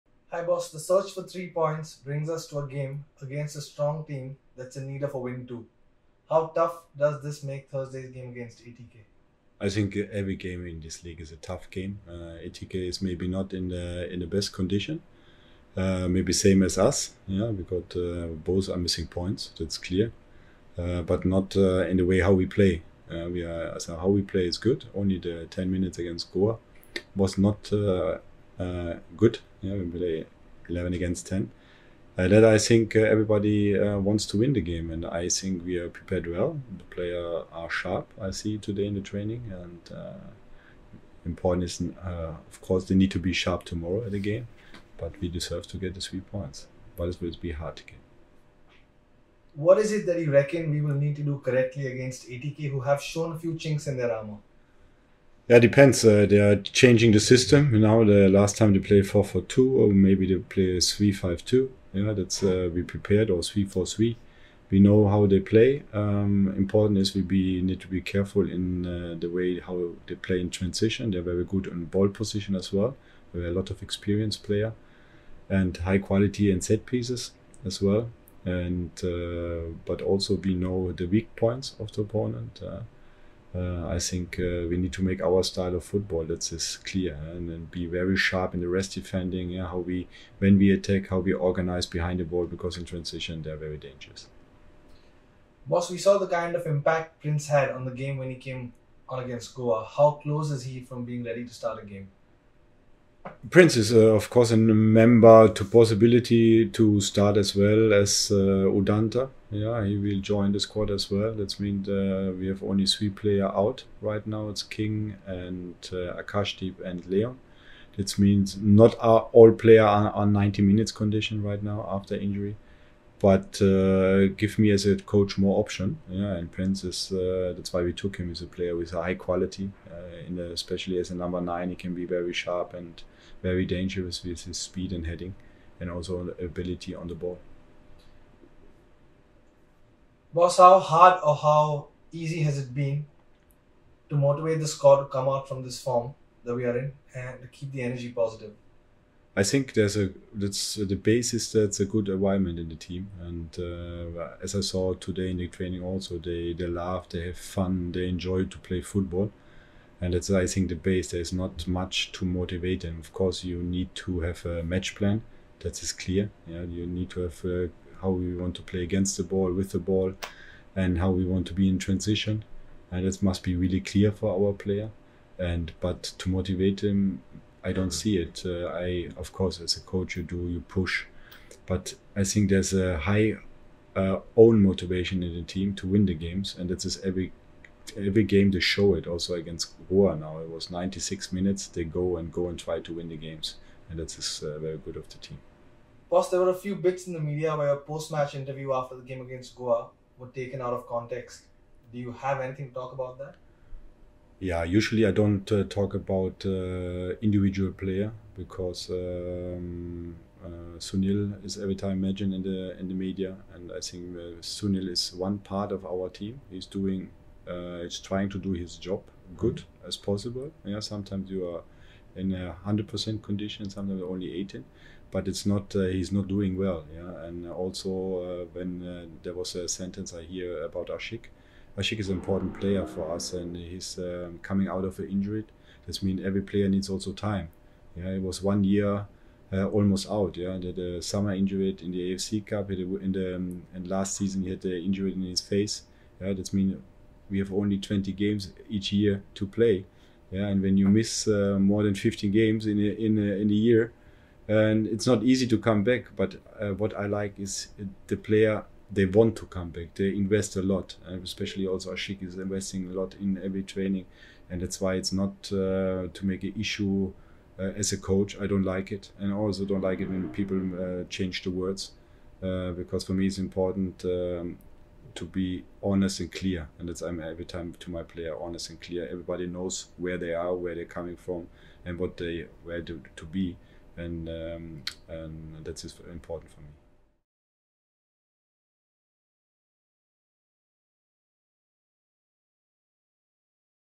Match Preview
We sat down with Blues' boss Marco Pezzaiuoli ahead of the big clash against ATK Mohun Bagan. Watch! ▶️